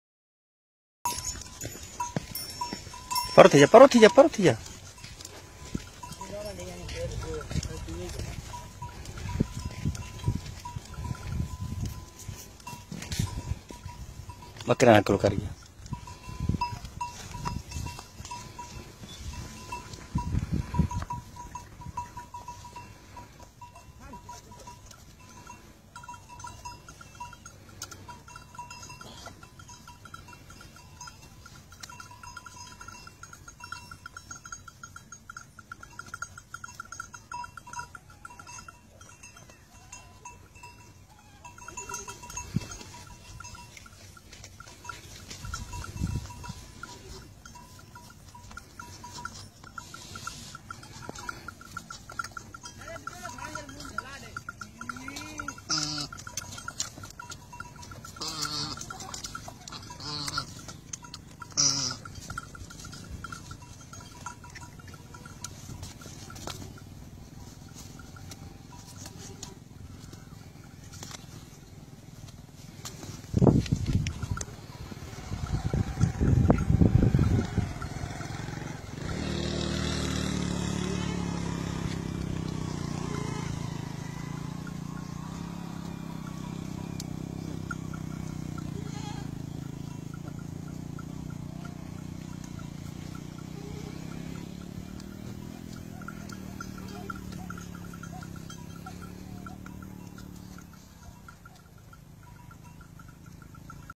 Animal love
Bakri aak harin ko dood palata haw
Aak gangli harin bakri ko dood pee rahi hai